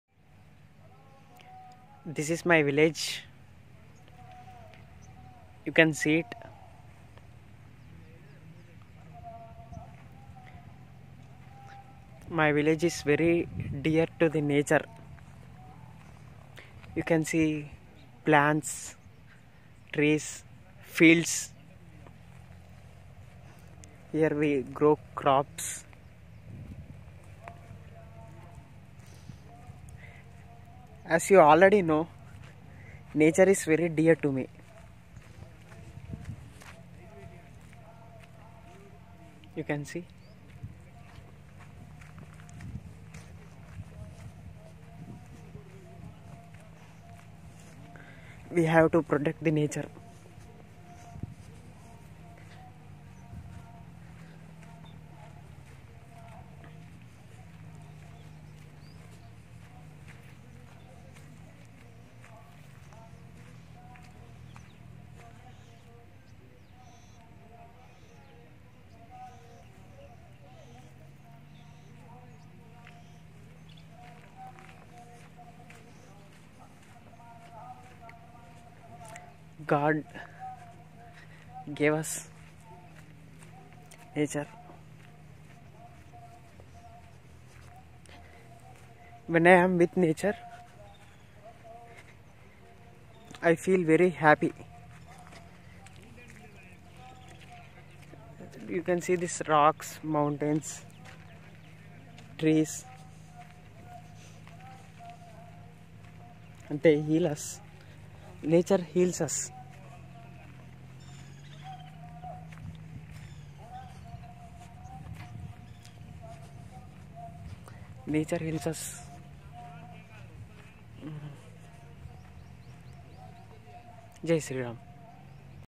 My Beautiful Village
Our village consists of beautiful farmlands, temples, hills and it is very close to nature. Water, land, and air are very pure. I like to spend my time on countryside farms.
My dream is to live in a peaceful countryside farmhouse listening to the glories of Bhagwan Ram. God created this beautiful nature, we had the responsibility to protect and preserve. Planting and protecting trees are very good qualities. Nature had many healing properties. Ancient saints lived very close to nature to get concentration for meditation. Living in a village is very advantageous in terms of spirituality and health. In our village, we get fresh milk, vegetables, fruits. My true happiness is chanting the glories of Bhagwan Ram under the shelter of nature.